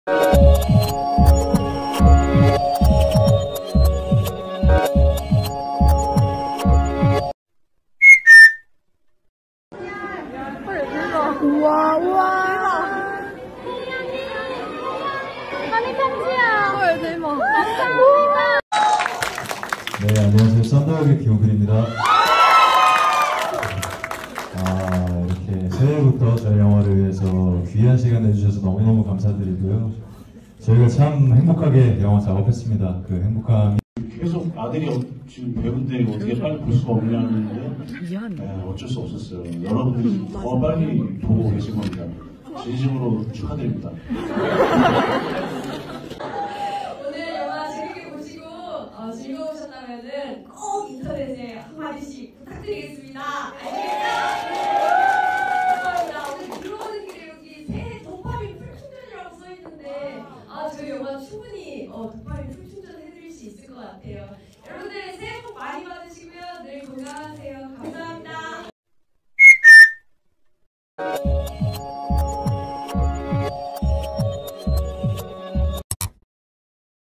240103 Min Woo Bin Kim Taeri & Ryu Jun Yeol at Movie Alienoid Part 2 Stage Greetings
Min Woo Bin Kim Taeri & Ryu Jun Yeol at Movie Alienoid Part 2 Stage Greetings

김태리 류준열 영화외계+인2 외계+인2

キムテリ